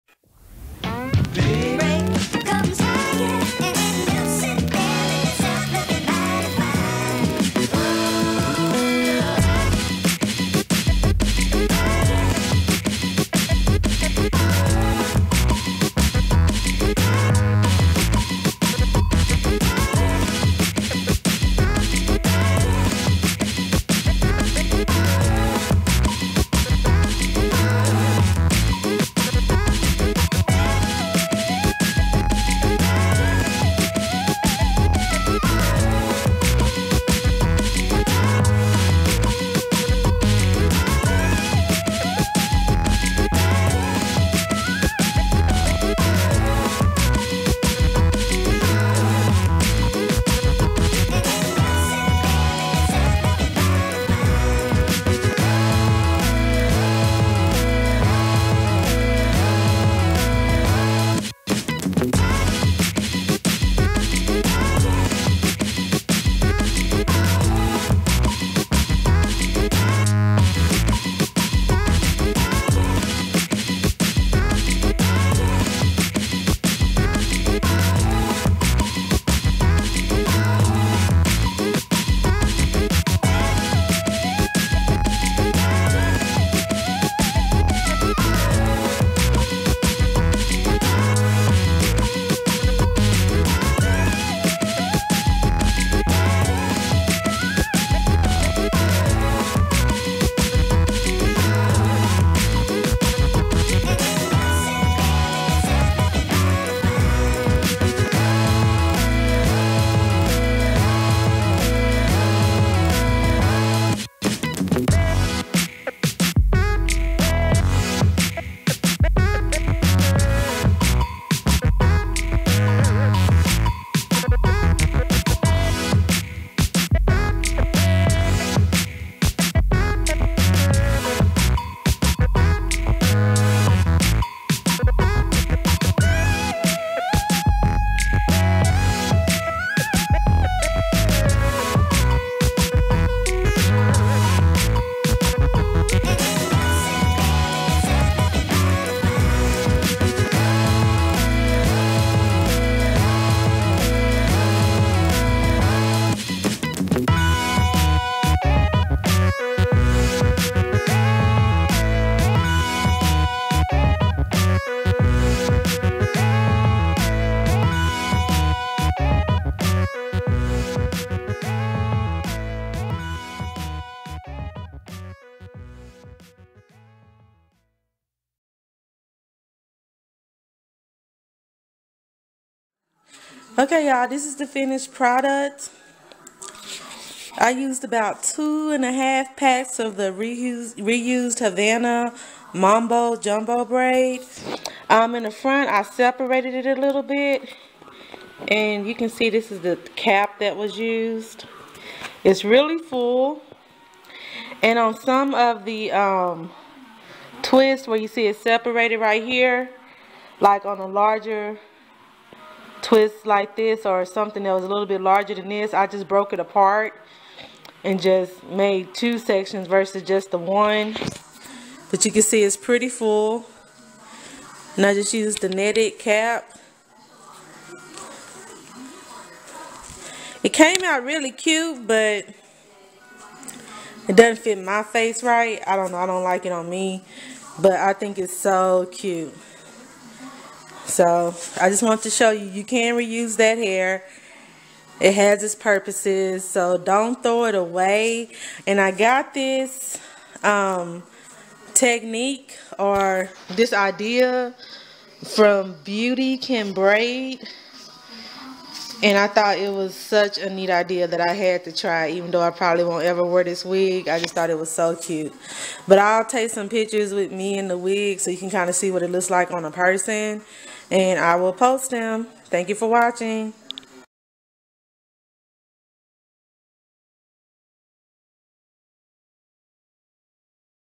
Crochet Wig: Recycled Havana Mambo Twist
This is not a tutorial, I'm just showing how I recycled the Havana twists. I think its a pretty neat idea.